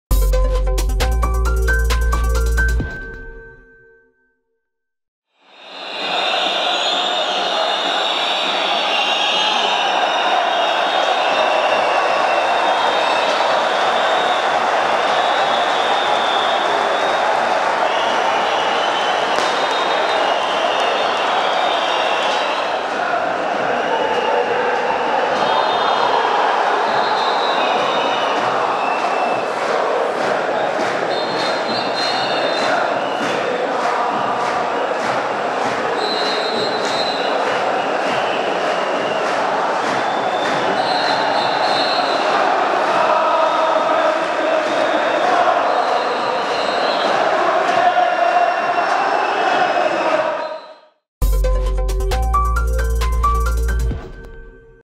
AS FAR-Raja: la belle illustration des supporters des Tricolores
Ambiance festive, des chants et de belles animations, les supporters de l’AS FAR se sont illustrés ce mardi soir, en brandissant un joli tifo, dans le virage sud du Complexe Moulay Abdellah de Rabat, baptisé "Royal", lors du clasico qui oppose les Militaires au Raja dans le cadre de la 29e journée de la Botola Pro.

يرجى الاشتراك..